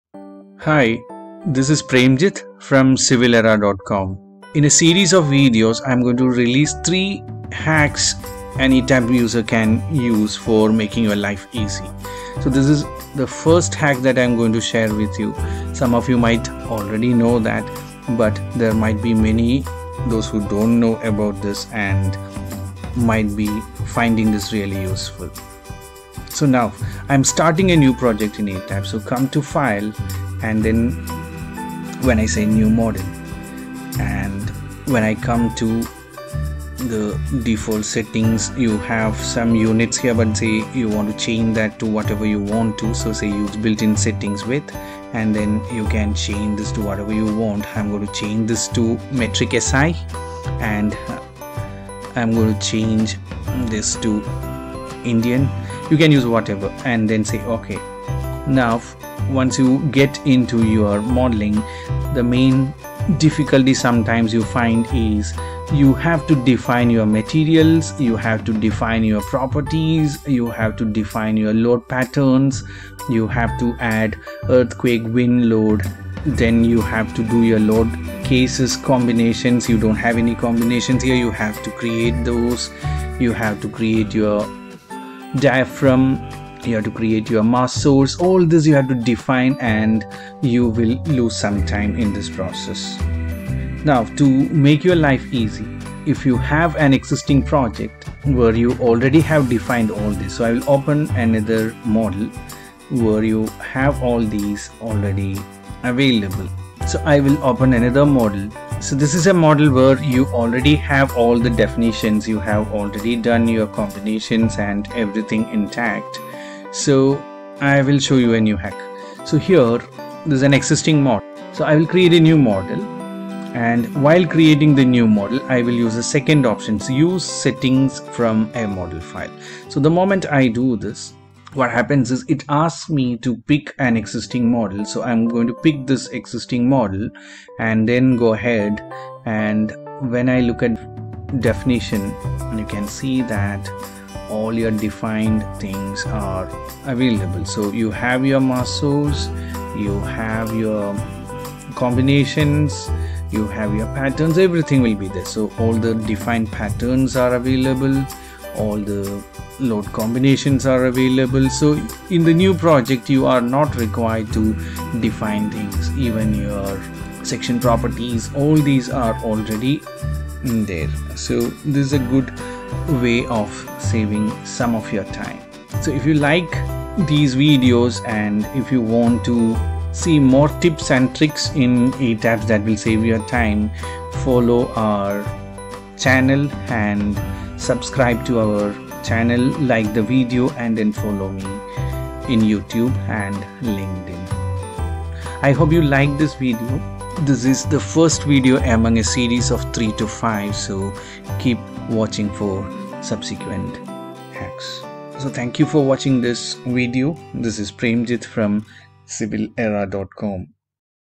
A quick hack in ETABS to ease your initialisation
Here is a quick hack that helps you speed up your ETABS modelling initialisation.
Some of you may be aware of this but then it helps many others who are new to this. I will be in a series of videos, discussing more such hacks that makes your etabs modelling easier.